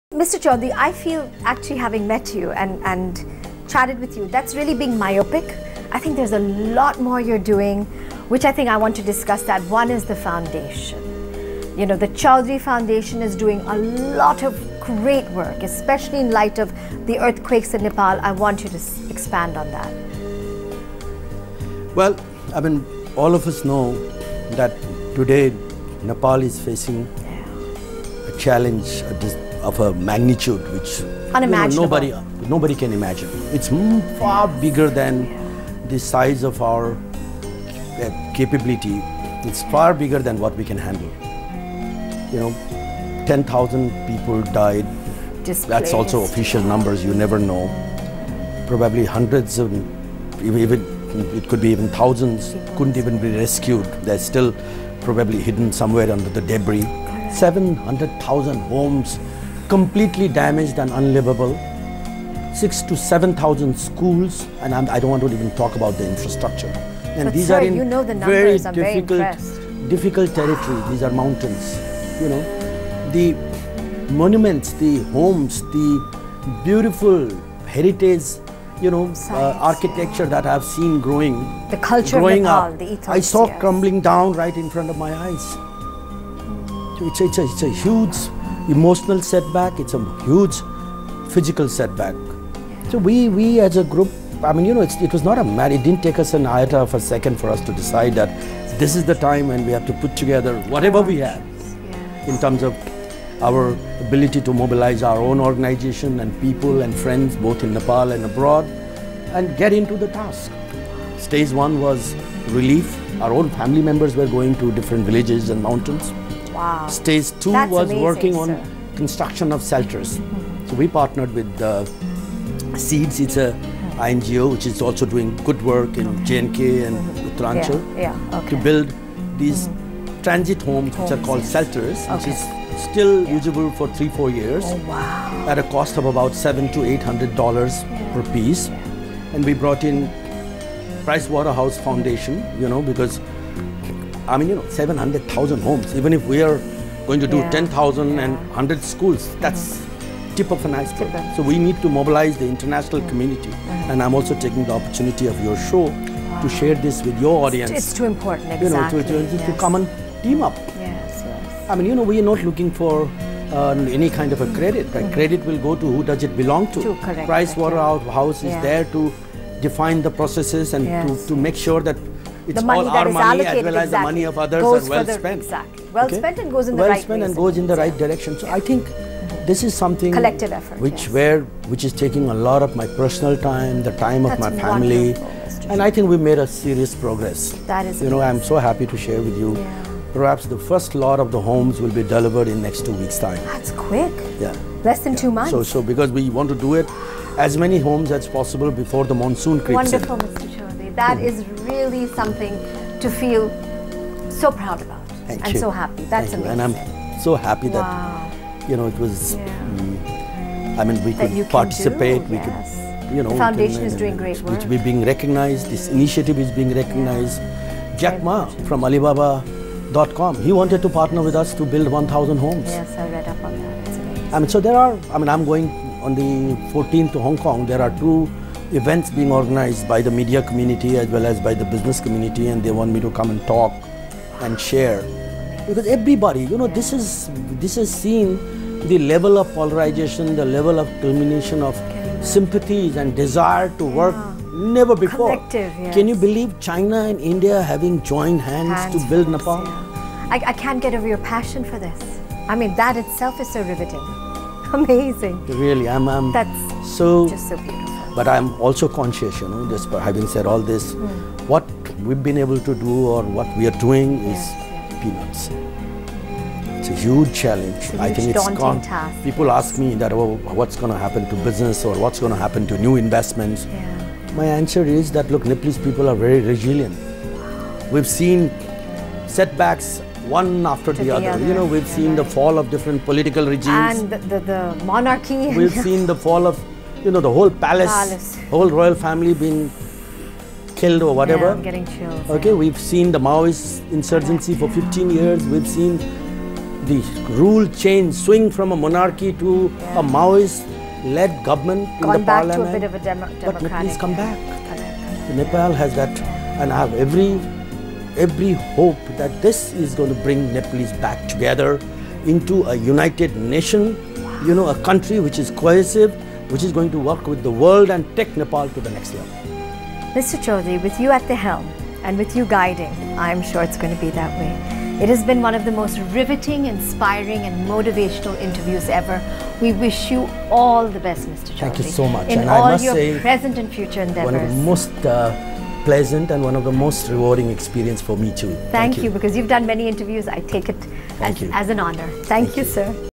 Conversation with philonthropist, businessman Binod Chaudhary, President, CG Global | Part-III
Conversation with philanthropist, businessman Binod Chaudhary, President, CG Global. Watch complete news story of for getting detailed updates!